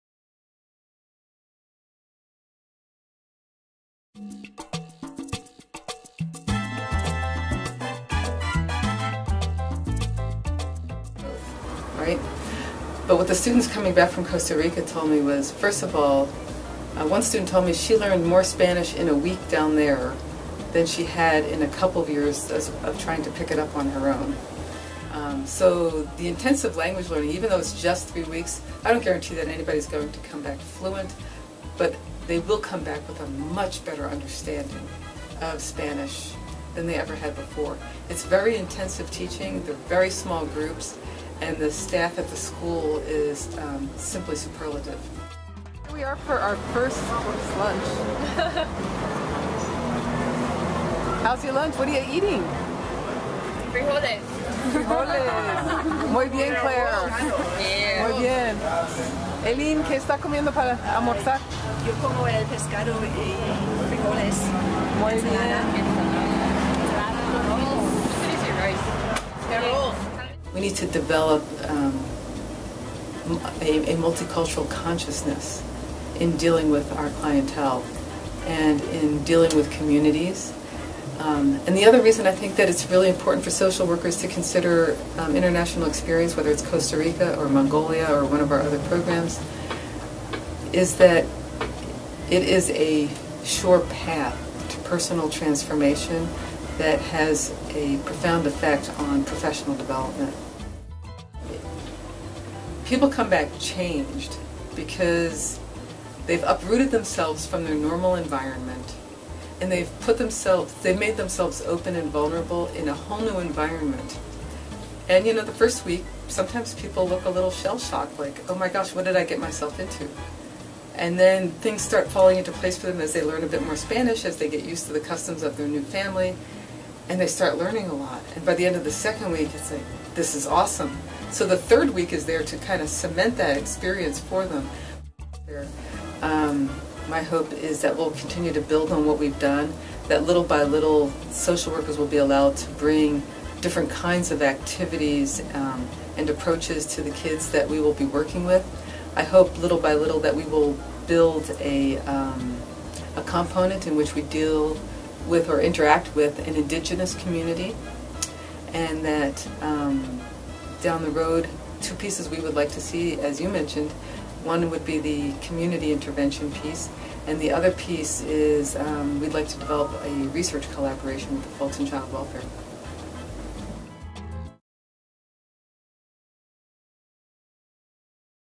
Study Abroad in Costa Rica
The University of Utah's College of Social Work and Department of Family and Consumer Studies host a three-week study abroad in beautiful San Jose, Costa Rica.  During the trip, students and community professionals engage in intensive Spanish language study, learn about a variety of social issues, gain culturally-appropriate interaction skills, and complete valuable community service projects.